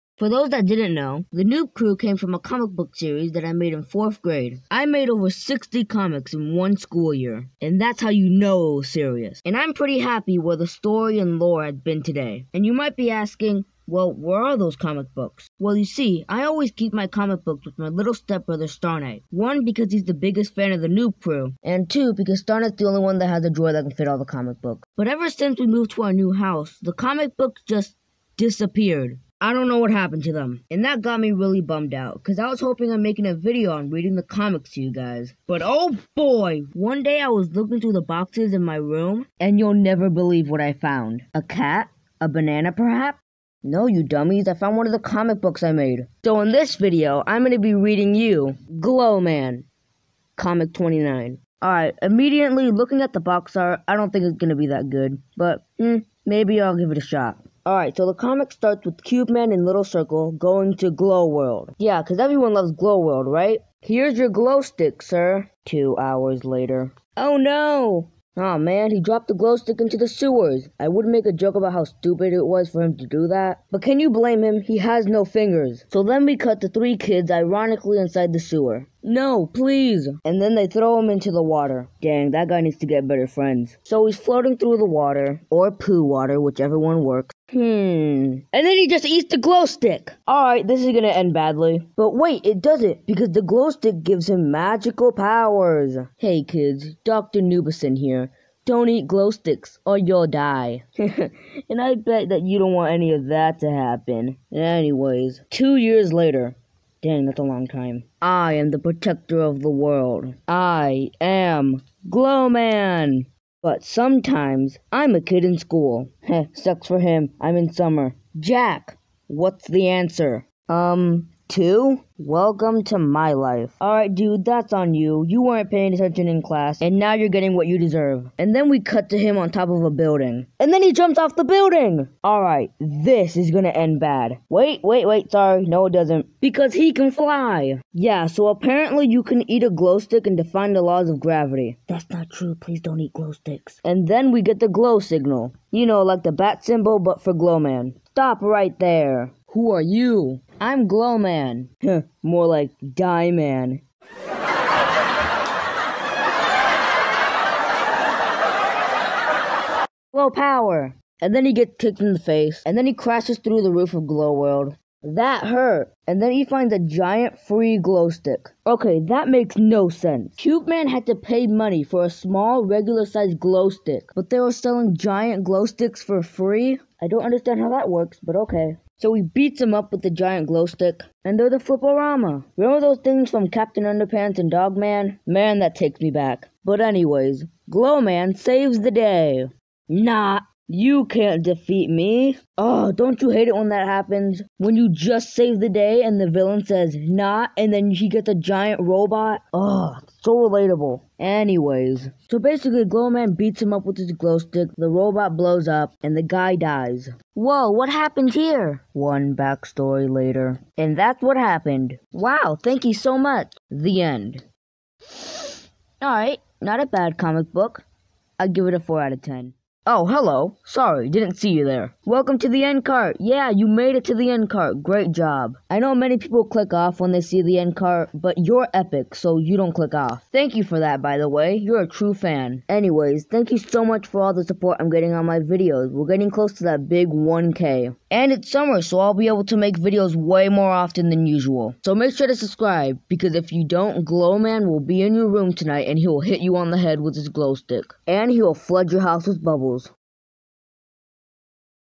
Comic I made in 4th grade
Hola my friends I’m made over 60 comics in 4th grade and I was only able to fine one...

Yes comic good for you and meeeeeeee